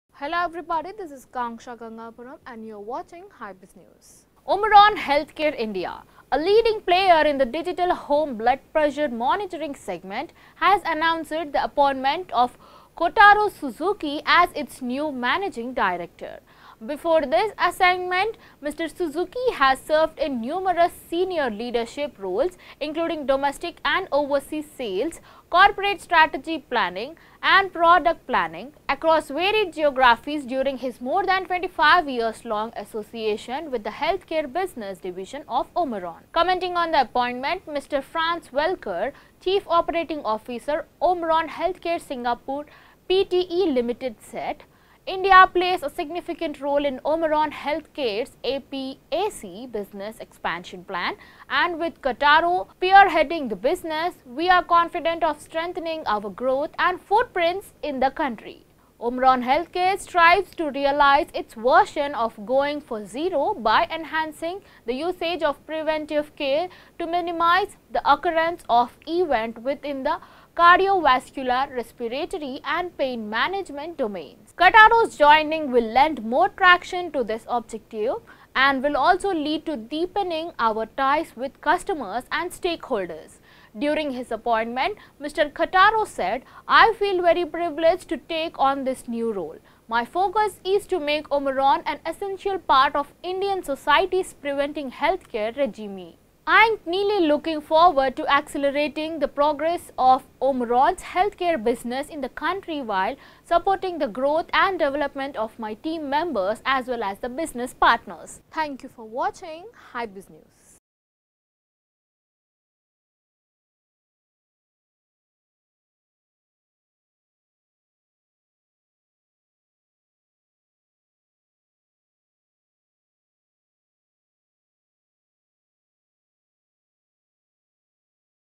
OMRON Healthcare India Appoints Kotaro Suzuki as MD || Hybiz tv
OMRON Healthcare India, a leading player in the digital home blood pressure monitoring segment, has announced the appointment of Kotaro Suzuki as its new Managing Director.
Before this assignment, Mr. Suzuki has served in numerous senior leadership roles including domestic & overseas sales, corporate strategy planning, and product planning, across varied geographies during his more than 25-years-long association with the healthcare business division of OMRON. Commenting on the appointment, Mr. Frans Velkers – Chief Operating Officer, OMRON Healthcare Singapore Pte Ltd said, “India plays a significant role in OMRON Healthcare’s APAC business expansion plan, and with Kotaro spearheading the business, we are confident of strengthening our growth & footprints in the country. OMRON Healthcare strives to realize its vision of “Going For Zero” by enhancing the usage of preventive care to minimize the occurrence of events within the cardiovascular, respiratory, and pain management domains. Kotaro’s joining will lend more traction to this objective and will also lead to deepening our ties with customers and stakeholders.”
During his appointment, Mr. Kotaro said, “I feel very privileged to take on this new role. My focus is to make OMRON an essential part of Indian society’s preventive healthcare regime. I am keenly looking forward to accelerating the progress of OMRON’s Healthcare business in the country while supporting the growth & development of my team members as well as business partners.”